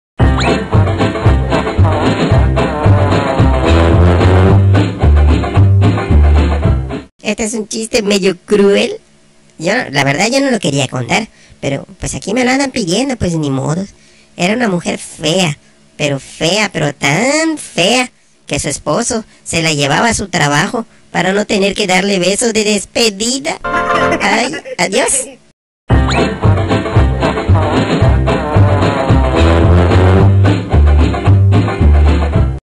QUE FEA ERA ESA SEÑORA GOYITO EL ARDILLO
EL ARDILLO GOYITO, nos cuenta un chiste muy bueno en su particular estilo de contarlo...que se diviertan